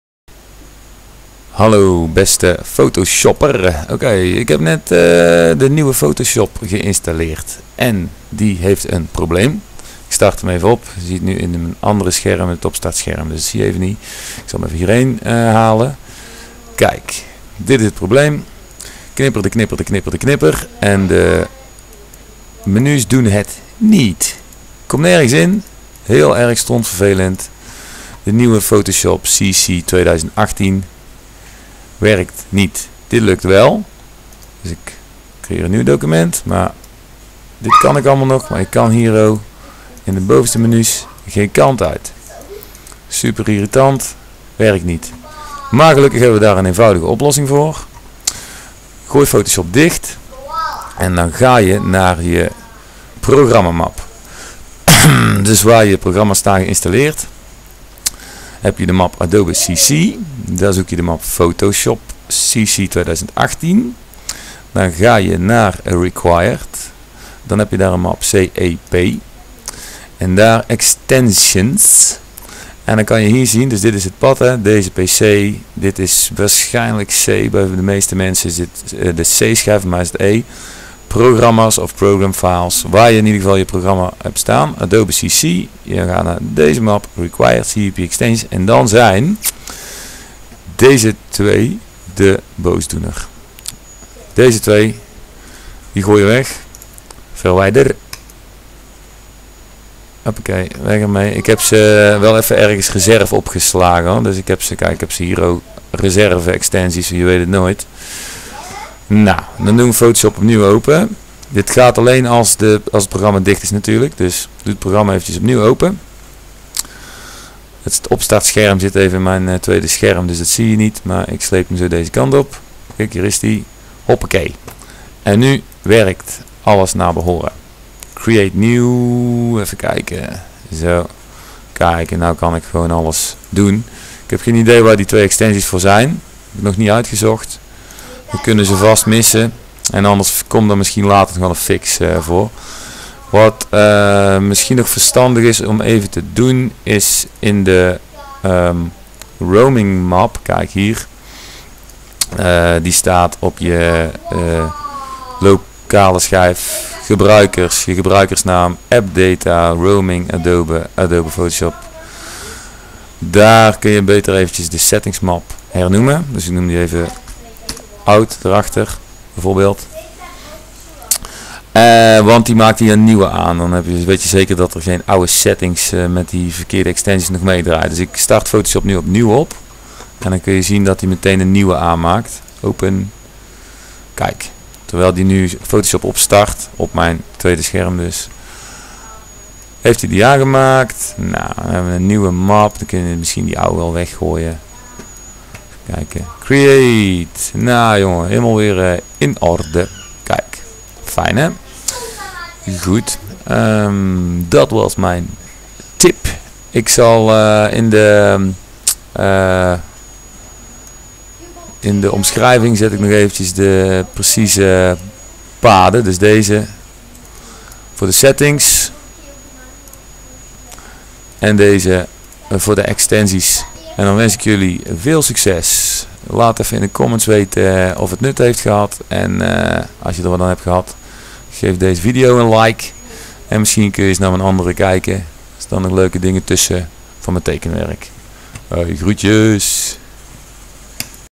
Photoshop CC 2018 Flickering cursor/menus not opening Problem Fix
An easy fix for the Flickering Cursor problem in the new Adobe Photoshop CC 2018. (Update october 2017) on Windows 10.

If you encounter this problem, this fix is for you:
When opening Photoshop CC 2018 the cursor keeps flickering, blinking or refreshing and you can't click the menus open.

It is caused by two extensions. If you remove these, your Photoshop installation works like a charm again.

Here is the description, the filenames and the paths to the right folders.
Watch the video to see this fix in action.

Close your Photoshop application

Go to

C:\Users\Username\AppData\Roaming\Adobe\Adobe Photoshop CC 2018

Rename the folder

"Adobe Photoshop CC 2018 Settings"
to
"Adobe Photoshop CC 2018 Settings.old" or "Adobe Photoshop CC 2018 Settings.crap" or "Adobe Photoshop CC 2018 Settings.thanksalotadobe"

Then go to:
C:\Programs\Adobe CC\Adobe Photoshop CC 2018\Required\CEP\extensions

(the path on your PC can differ. It could be E:\Programma's or something. Just find the folder with the files "Adobe CC\Adobe Photoshop CC 2018\Required\CEP\extensions" in it and you're good.

In this folder remove the extensions:

com.adobe.Butler.backend
and
com.adobe.DesignLibraryPanel.html

If you 're smart you back-up them somewhere (just in case) before you remove them.

For Adobe Premiere 2018:
Remove icudtl.dat from the CEPHtmlEngine folder.
E:\Programma's\Adobe CC\Adobe Premiere Pro CC 2018\CEPHtmlEngine

E:\Programma's\ is probably different at your PC, just find the directory where Premiere is installed in.

For Illustrator: Remve icudtl.dat from the CEPHtmlEngine folder. You find it here: C:\Programma's\Adobe CC\Adobe Illustrator CC 2018\Support Files\Contents\Windows\CEPHtmlEngine

In Indesign zit ie hier: E:\Programma's\Adobe CC\Adobe InDesign CC 2018\Resources\CEP\CEPHtmlEngine Dus daar naartoe en "icudtl.dat" eruit mikken. Nu werkt Indesign naar behoren alleen kun je zo geen documenten aanmaken dus je moet even je settings aanpassen. Met CTRL-K open je je settings. Dan ga je naar General en vink je "Show Start workspace when no documents are open" uit en "Use Legacy New Document Dialog" aan.

Success!

Visit my other videos to see how I draw my cartoons or vist my

IMPORTANT!!
You know what also works? And makes this entire video superfluous... Just install your CC apps on your C drive. Then everything just works as intended. Seriously.